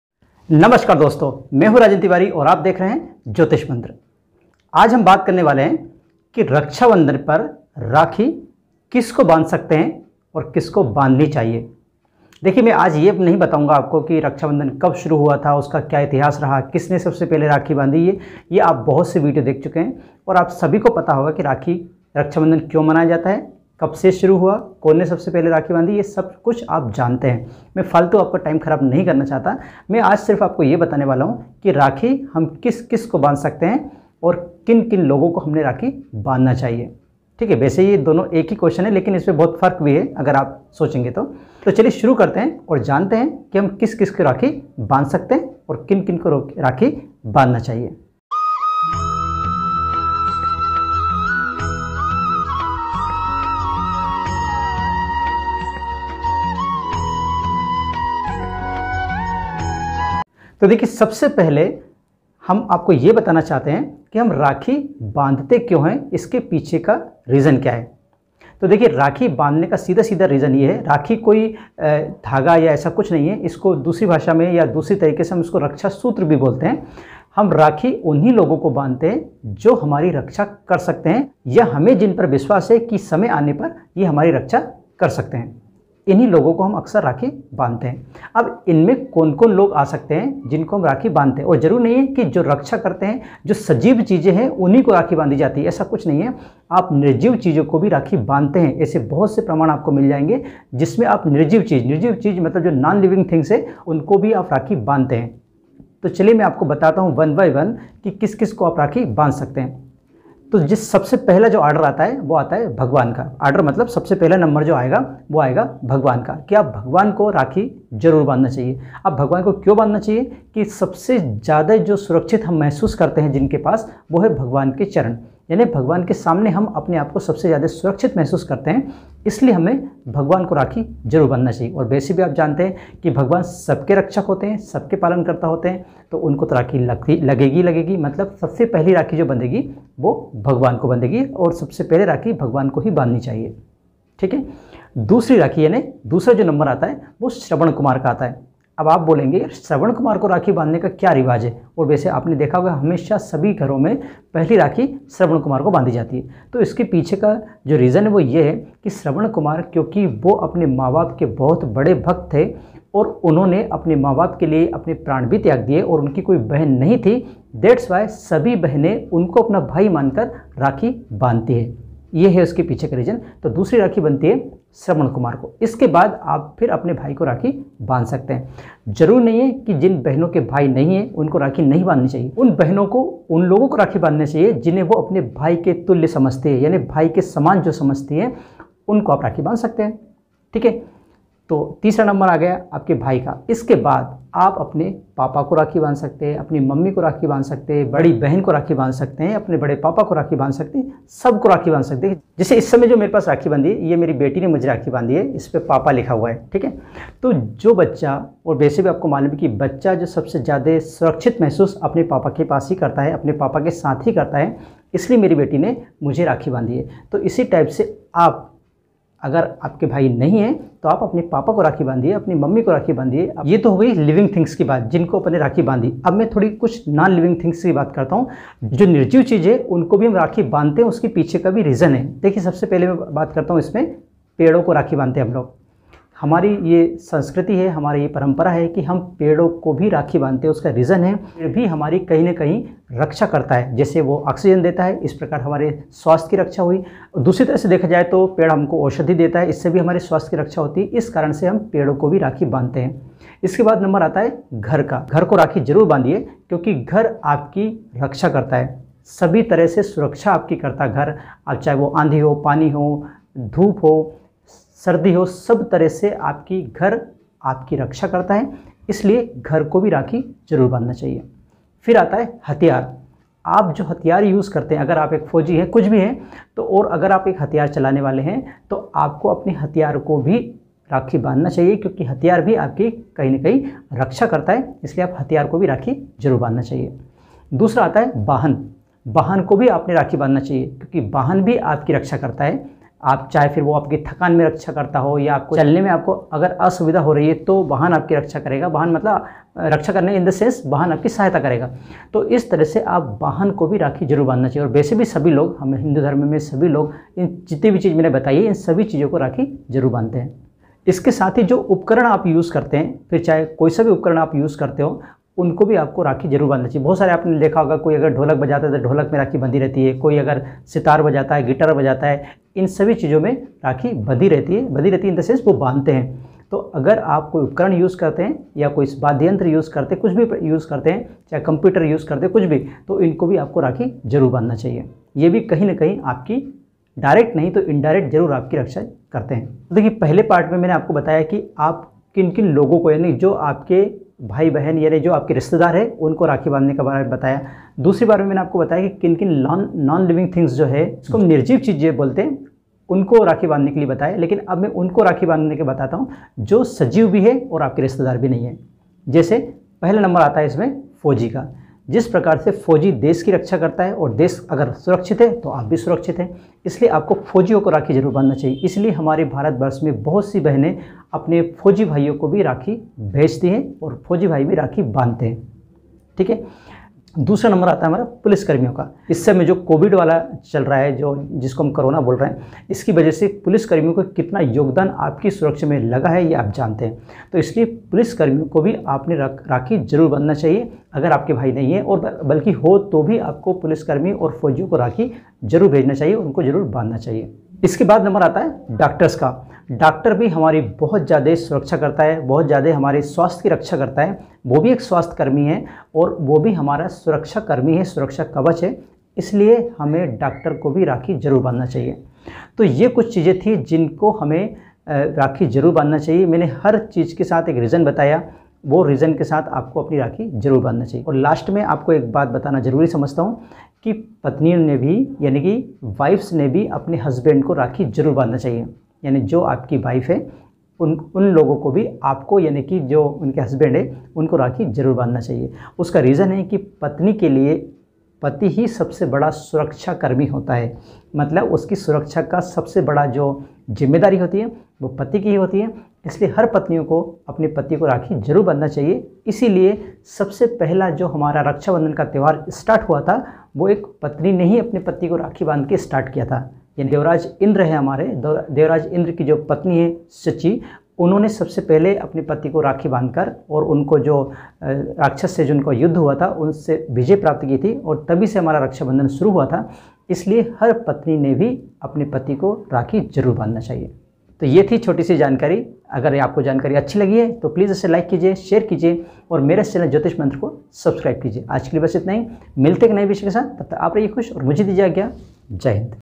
राखी किस किस को बांध सकते है।राखी बांधने के लिए क्या है जरूरी शर्त।।
नमस्कार दोस्तों जैसे कि आपको पता है कि आज रक्षा बंधन